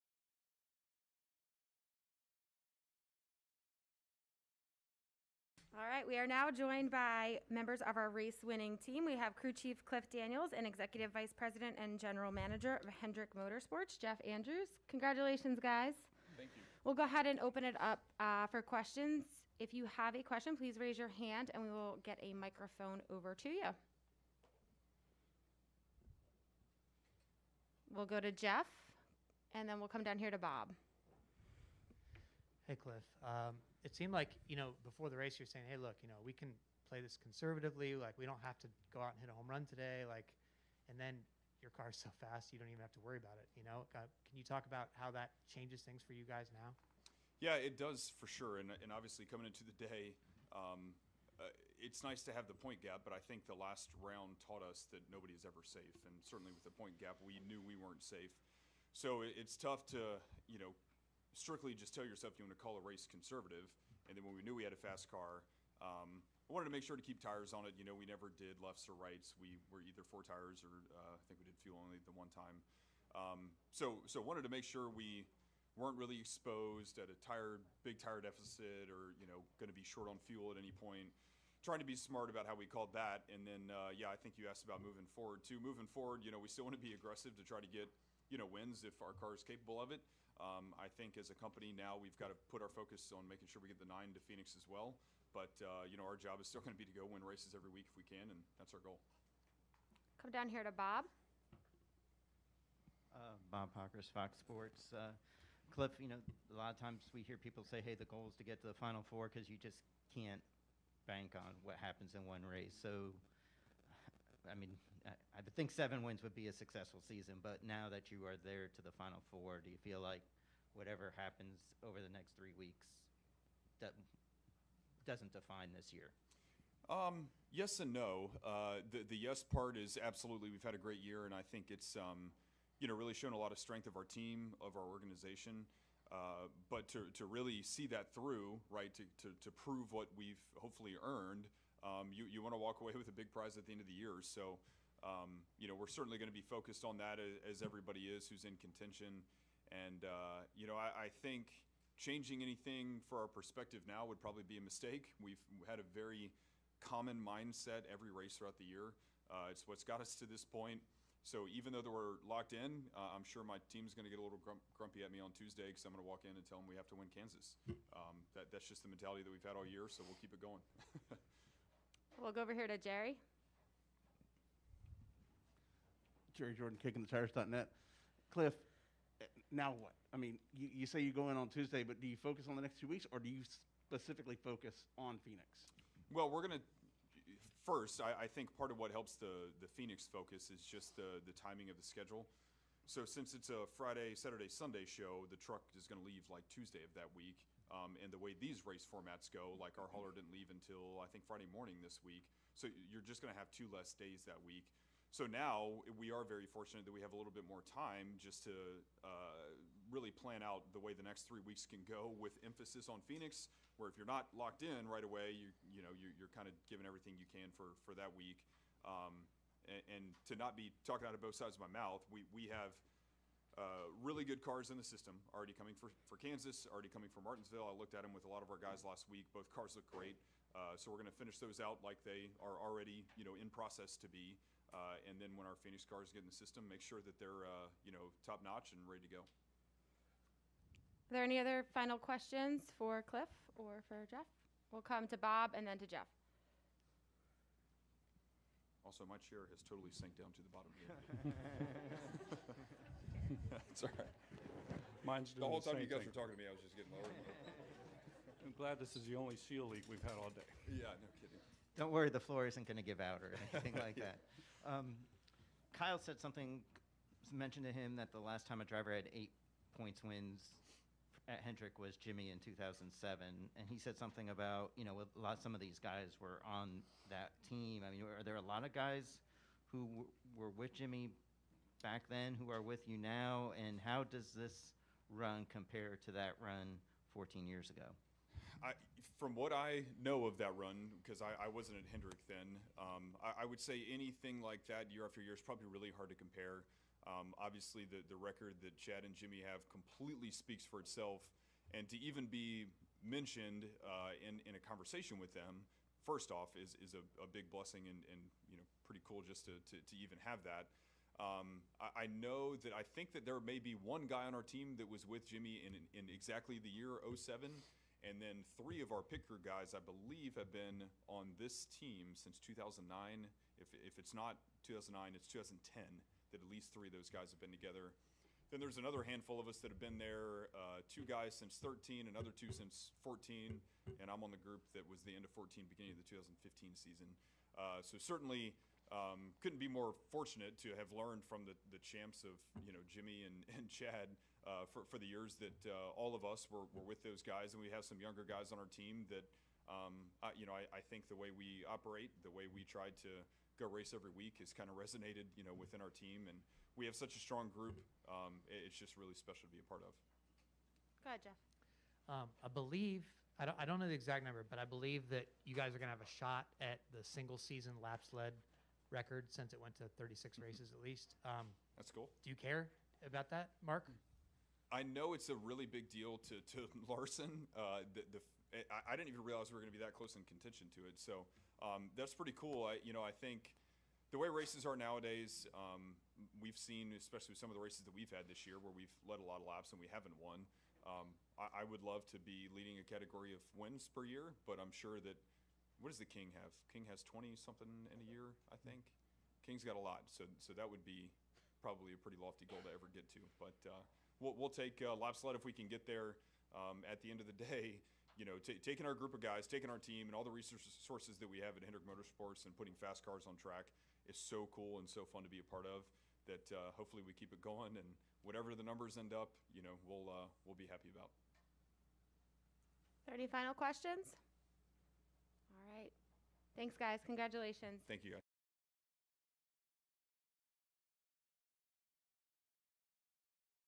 NASCAR at Texas Motor Speedway Oct. 2021: Cliff Daniels post race
Full post race Zoom with Hendrick Motorsports executive Jeff Andrews and winning crew chief Cliff Daniels at Texas Motor Speedway Sunday Oct. 17, 2021.